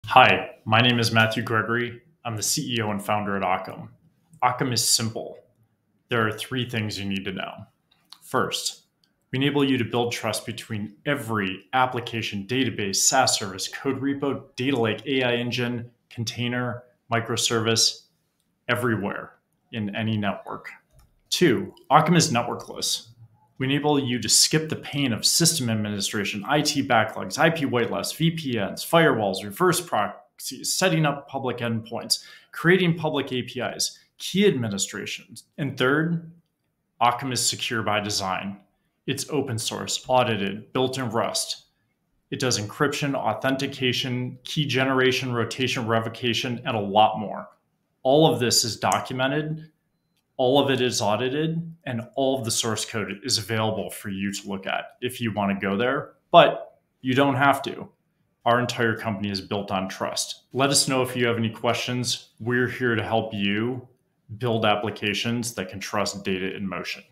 What is Ockam?
Ockam is simple.

There are three things you need to know.

1) We enable you to build trust between every application, database, SaaS service, code repo, datalake, AI engine, container, microservice, everywhere, in any network.

2) Ockam is networkless. We enable you to skip the pain of system administration, IT backlogs, IP white lists, VPNs, firewalls, reverse proxies, setting up public endpoints, creating public APIs, key administration.

3) Ockam is secure by design. It's open source, audited, built in Rust. It does encryption, authentication, key generation, rotation, revocation, and a lot more. All of this is documented, all of it is audited, and all of the source code is available for you to look at if you want to go there, but you don't have to.

Our entire company is built on Trust.

Let us know if you have any questions. We are here to help you build applications that can trust data in motion. It's that simple.